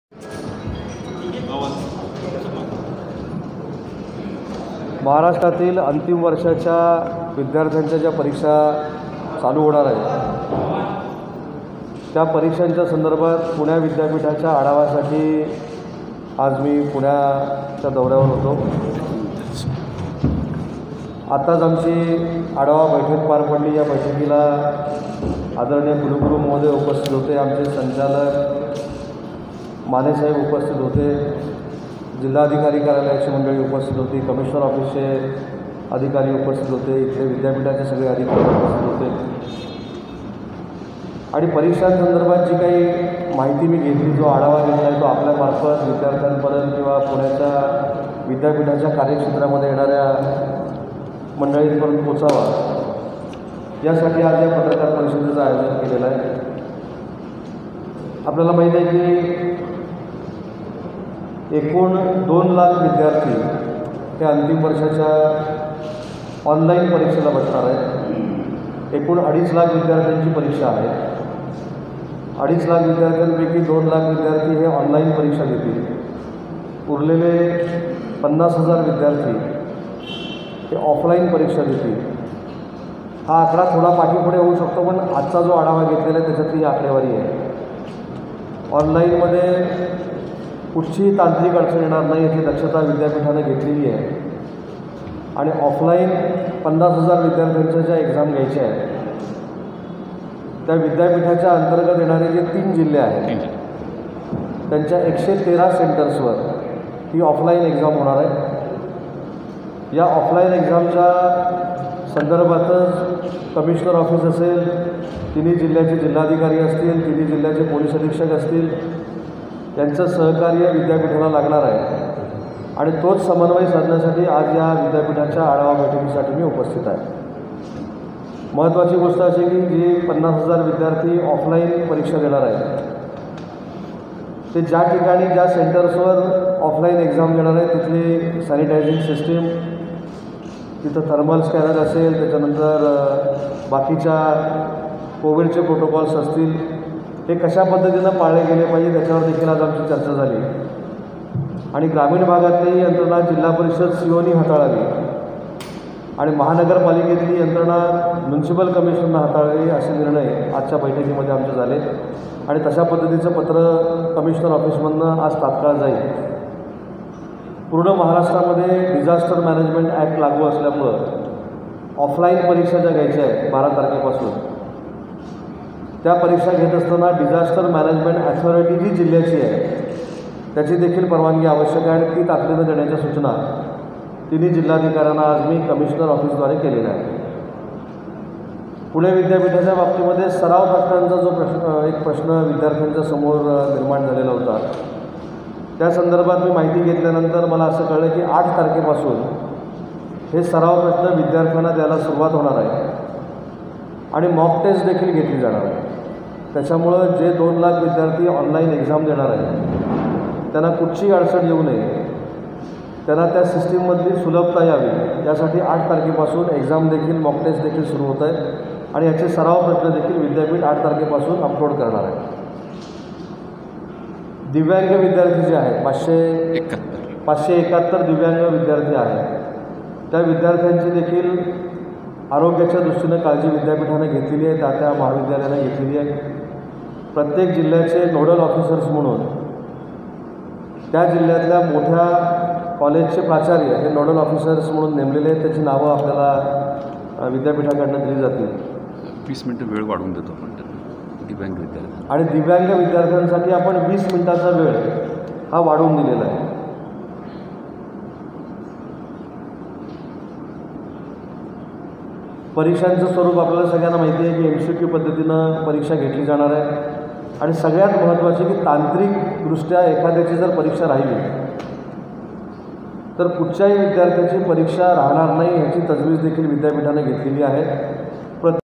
विद्यार्थ्यांच्या आरोग्याची काळजी घेत अंतिम वर्षाच्या परिक्षा घेण्यात येणार - उदय सामंत
पुणे - पुणे विद्यापिठा अंतर्गत एकूण अडीच लाख विद्यार्थी अंतिम वर्षाची परीक्षा देणार आहेत. त्यापैकी दोन लाख विद्यार्थी हे ऑनलाईन परीक्षा तर 50 हजार विद्यार्थी ऑफलाईन परीक्षा देणार आहेत, कोविड-19 च्या पार्श्वभूमीवर सर्व नियमांचे पालन करून विद्यार्थ्यांच्या आरोग्याची काळजी घेत अंतिम वर्षाच्या परीक्षा घेण्यात येणार असल्याचे  उच्च व तंत्रशिक्षण मंत्री उदय सामंत यांनी  सांगितले. एकही विद्यार्थी परीक्षेपासून वंचित राहणार नाही, असा विश्वासही त्यांनी व्यक्त केला.